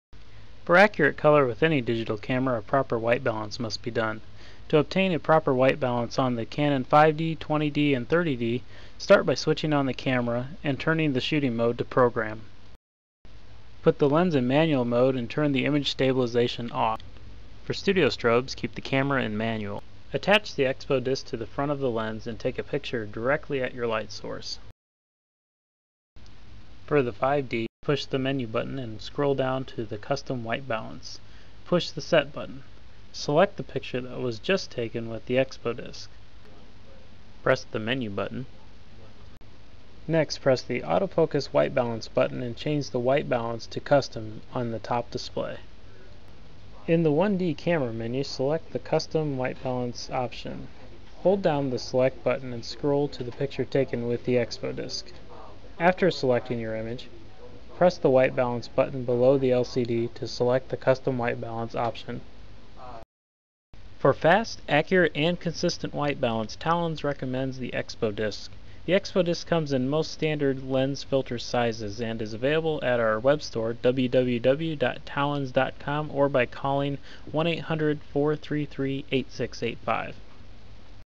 How to Obtain a White Balance on the Canon EOS Cameras
How to obtain a proper custom white balance on the Canon Eos Camera. Demonstrated with an Expodisc.